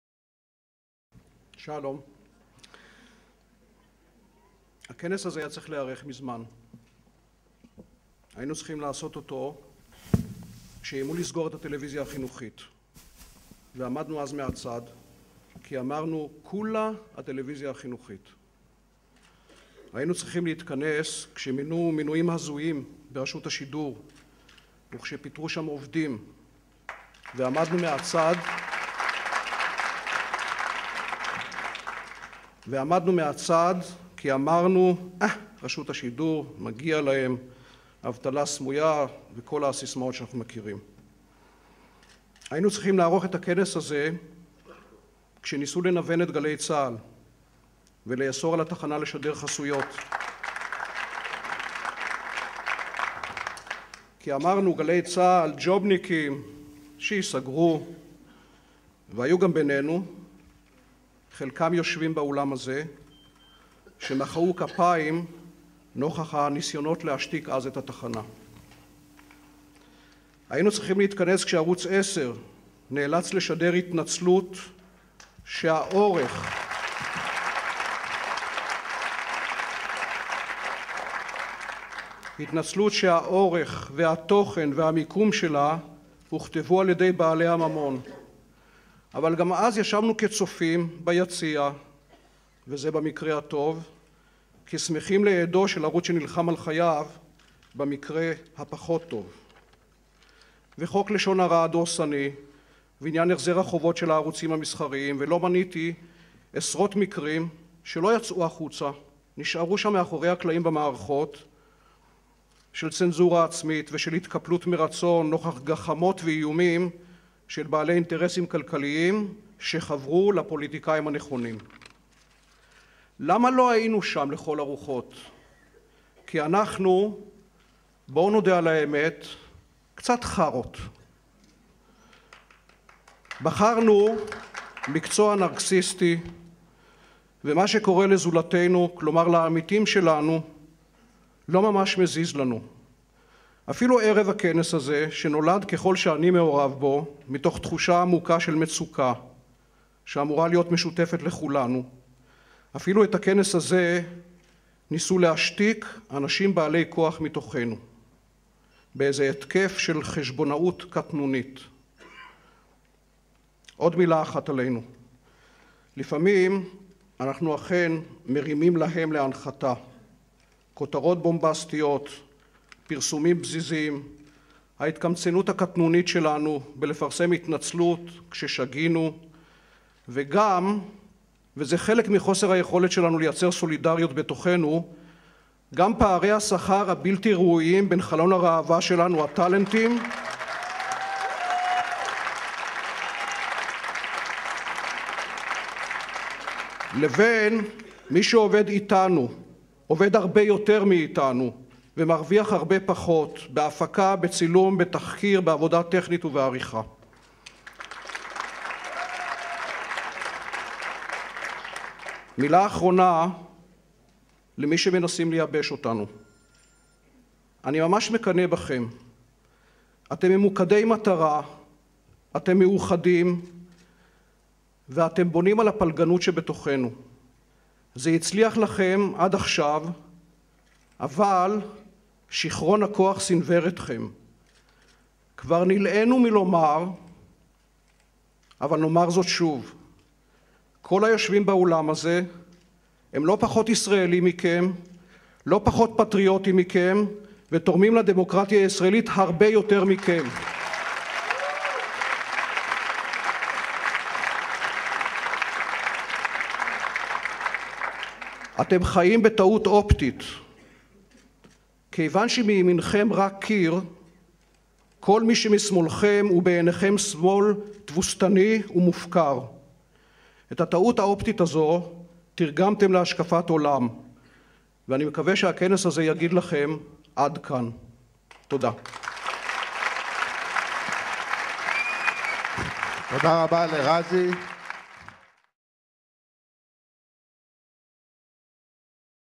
רזי ברקאי - כנס חירום לחופש העיתונות
אנשי תקשורת בכינוס חירום בשל חקיקה המגבילה את חופש הביטוי ובשל האיומים על רשות השידור ועל ערוץ 10. דבריו של העיתונאי  רזי ברקאי.